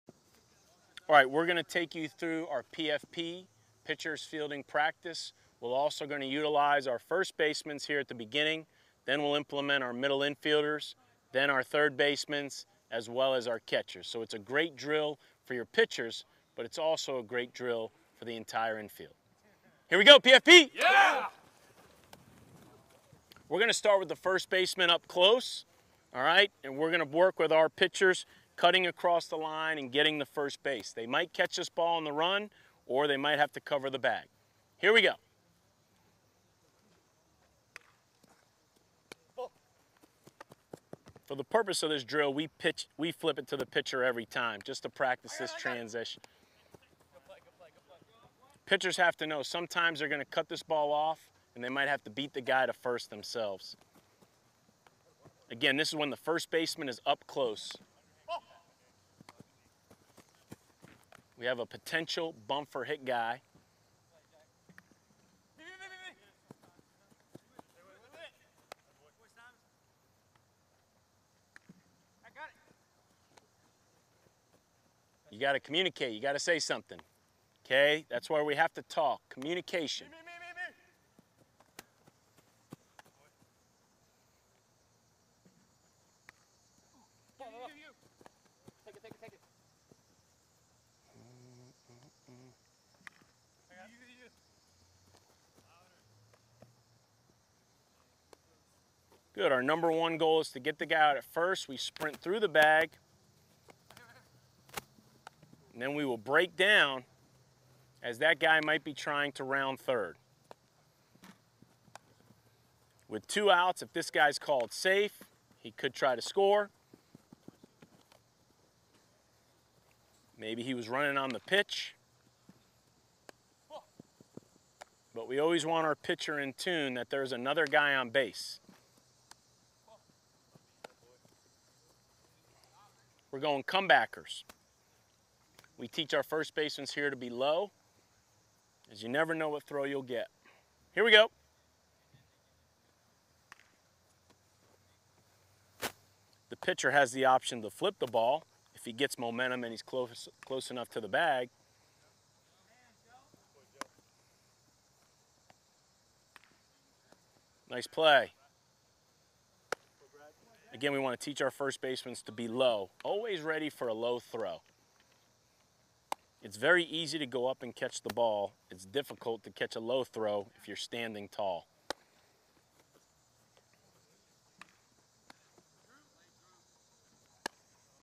All Access Louisville Baseball Practice
Dan McDonnell uses live practice footage to take you inside one of the best collegiate baseball programs in the country. Watch as the pitchers work their PFP drills while Coach McDonnell shares coaching points for each movement. A number of PFP drills are offered, covering everything from ground balls around the entire infield, comebackers, pick offs, and more.